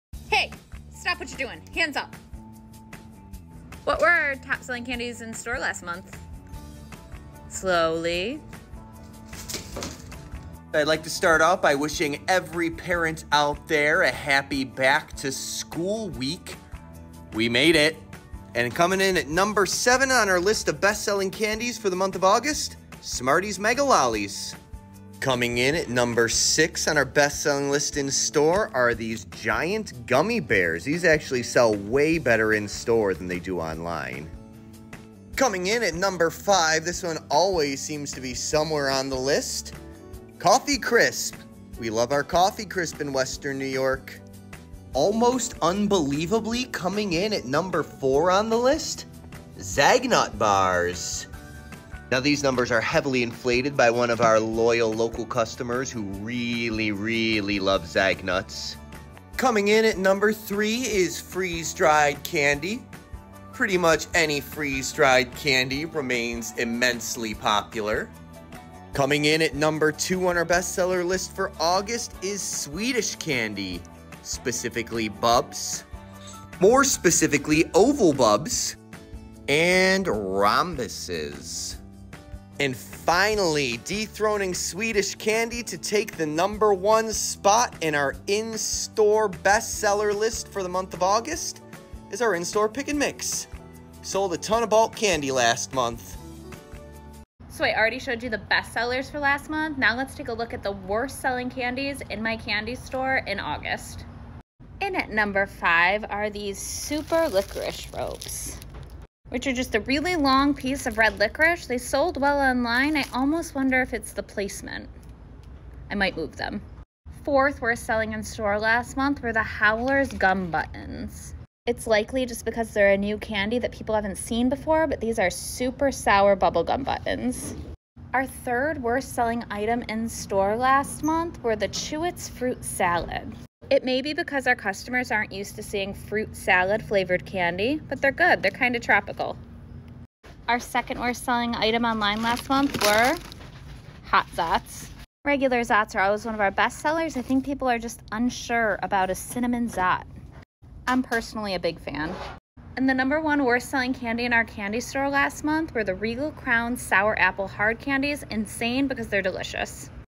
Best and Worst Selling Candies in Our Candy Store | Hello Sweets Candy Best Sellers Worst Sellers
Here were the best and worst sellers in our candy store for the month of August 2024!